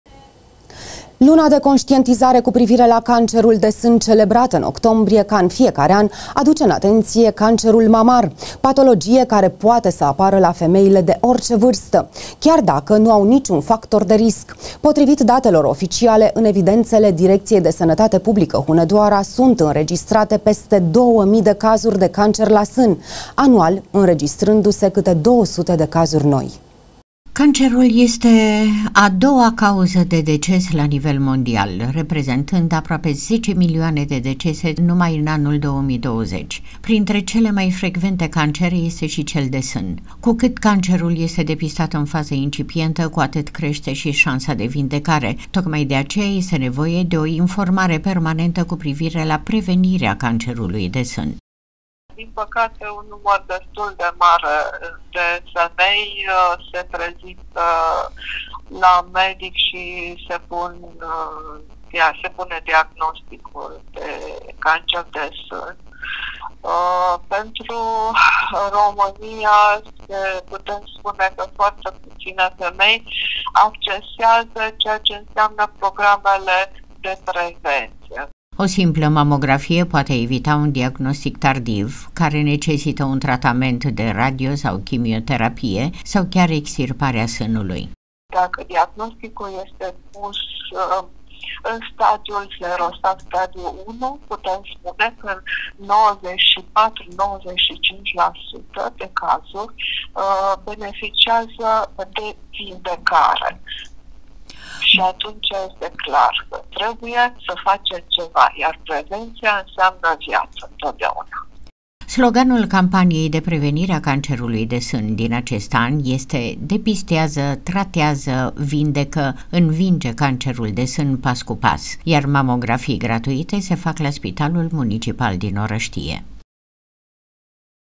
OCTOMBRIE, LUNA DE CONȘTIENTIZARE PRIVIND CANCERUL DE SÂN
CAMPANIA: „DEPISTEAZĂ. TRATEAZĂ. VINDECĂ. ÎNVINGE CANCERUL DE SÂN, PAS CU PAS!”
     Luna de conștientizare cu privire la cancerul de sân, celebrată în octombrie, în fiecare an, aduce în atenție cancerul mamar, patologie care poate să apară la femeile de orice vârstă, chiar dacă nu au nici un factor se risc. Potrivit datelor oficiale, în evidențele Direcției de Sănătate Publică Hunedoara sunt înregistrate peste 2.000 de cazuri de cancer la sân, anual înregistrându-se câte 200 de cazuri noi.